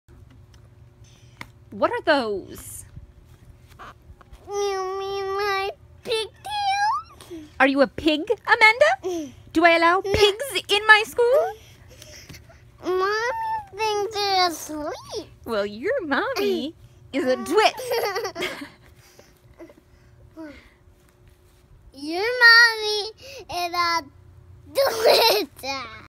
London’s Favorite Scene from “Matilda”
London and I reenacting Miss Trunchbull and Amanda Thripp’s scene from “Matilda”